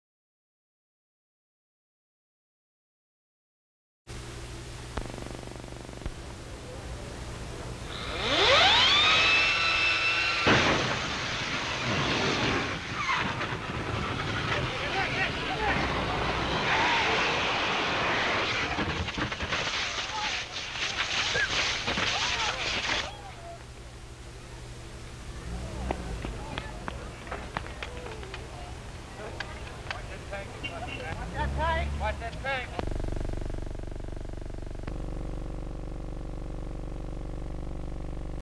A triphibian aircraft catches fire during its test run in Washington DC. HD Stock Footage
A triphibian aircraft catches fire during its test run in Washington DC.

An early mishap in the history of flight: An unusual triphibian aircraft attempting takeoff in Washington DC. US Government building along the side of a street. The airplane, not an ornithopter like some early designs, instead looks like a car with a flying saucer on top. It is seen moving on the street during its test run. The US Capitol in the background. The airplane quickly catches fire and burns. The pilot is taken out of the airplane before the fire has fully erupted. People stand near the wounded man. The wounded man lying in a hospital bed. Bandaged face of the man. A woman nursing his burns is sitting nearby. Location: Washington DC. Date: October 28, 1935.

57,000+ broadcast-quality historic clips for immediate download.